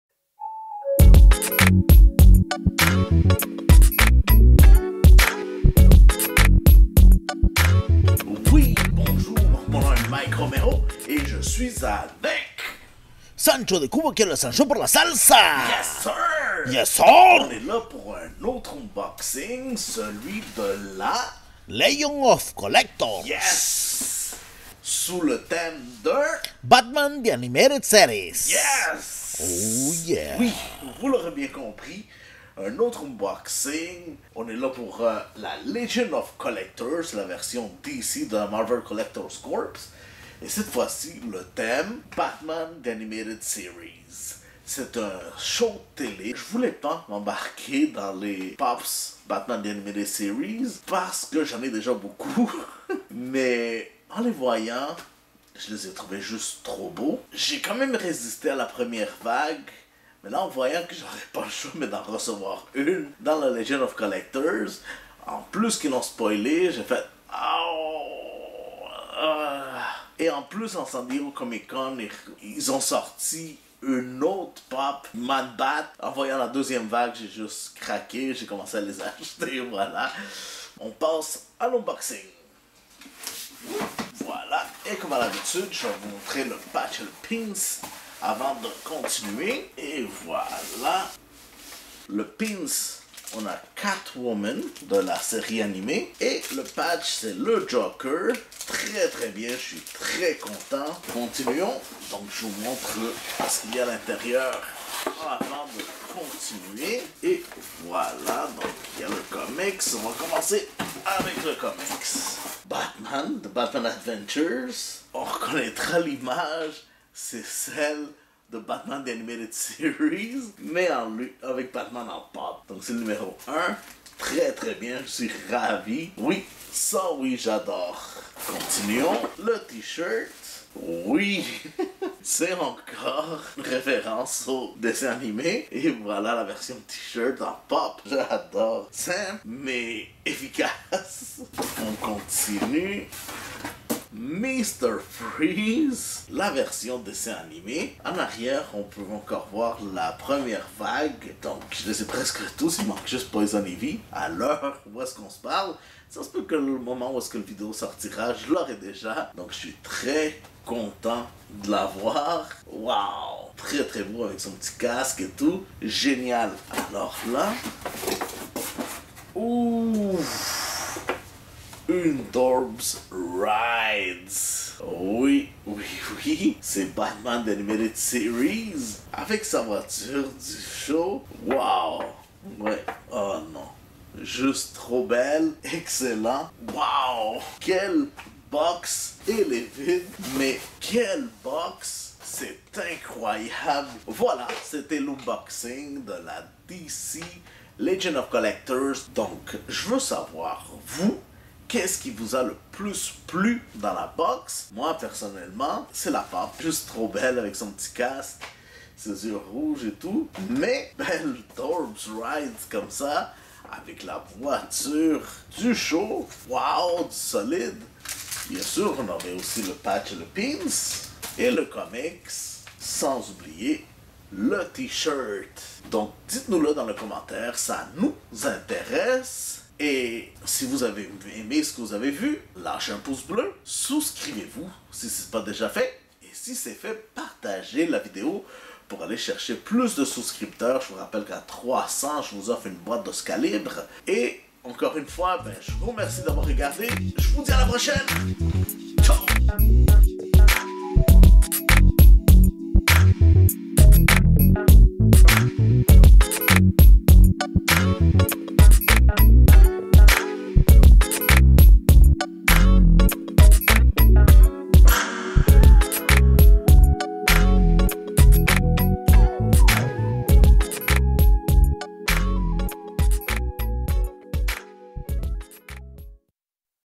Unboxing 70: Legion of Collectors - Batman: The Animated Series [FR]
Unboxing 70

Legion of Collectors - Batman: The Animated Series

Une boîte mystère sous le thème "Magie, Sorcellerie & Voodoo" où je garantis une Funko POP!, un T-Shirt et un comic. (3-6 items pour une valeur de 50-65$cad) +Article BONUS!

PS: Giveaway gratuit si remise en mains propres sinon frais de port à vos frais.